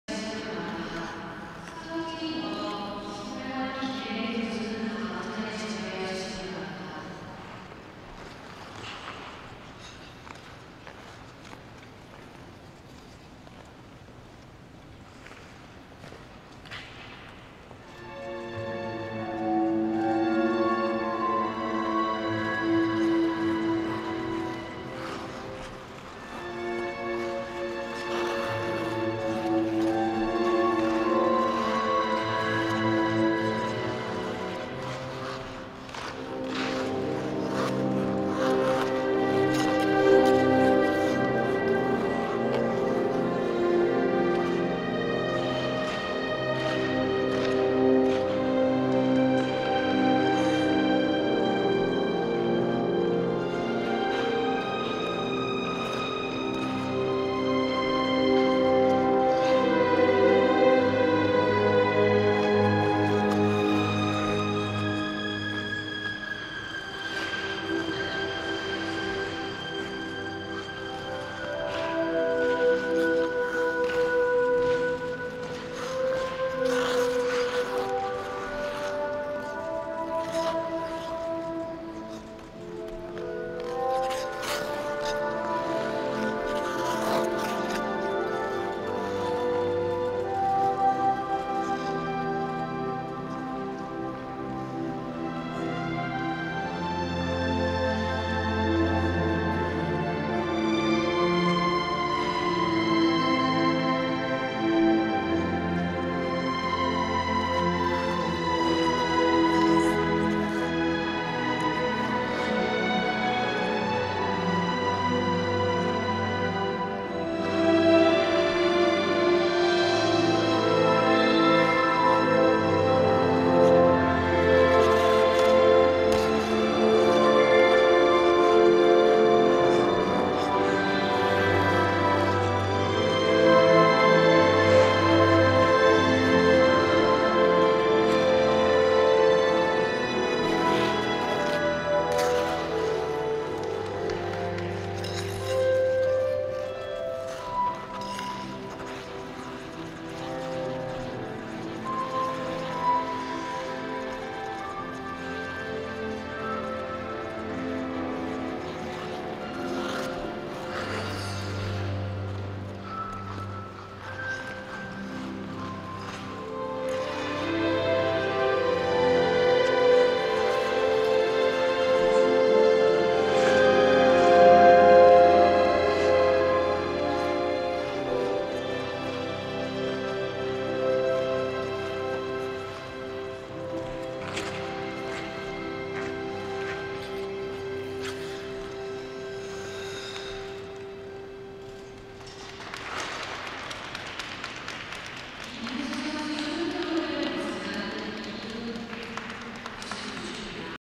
EUNSOO LIM 임은수,한강중3 @ 2018년 피겨 랭킹대회 공식연습 1일차 여자시니어 SP #27
2018/12/19, 목동 아이스링크, 2018년 전국남녀 피겨스케이팅 회장배 랭킹대회 공식연습 1일차

* 본 영상을 추출해서 동영상 사이트에 재업로드하는 행위는 금지합니다.